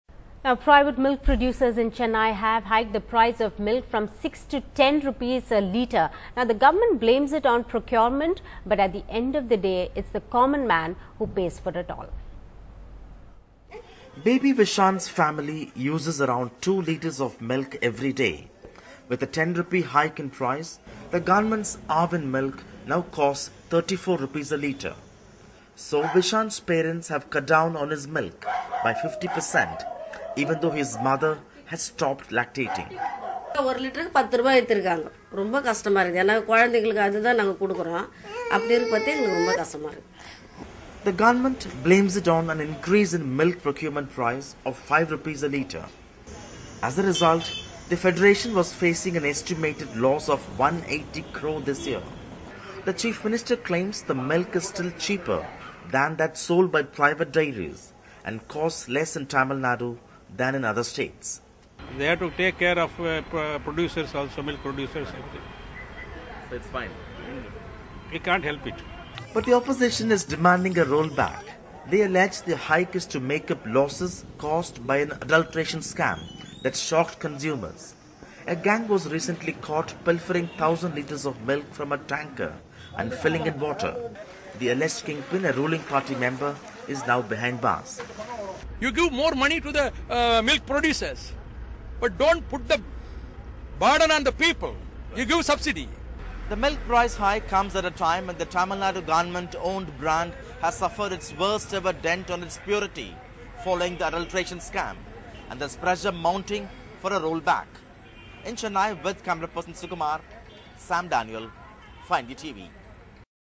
Milk price hike in Tamil Nadu may keep children from weaker sections hungry
The Tamil Nadu government's steep hike in milk price - Rs. 10 a litre - is expected to hit hard children from the economically weaker sections. Milk by the government agency Aavin, which was sold at Rs. 24 a litre, now costs Rs. 34.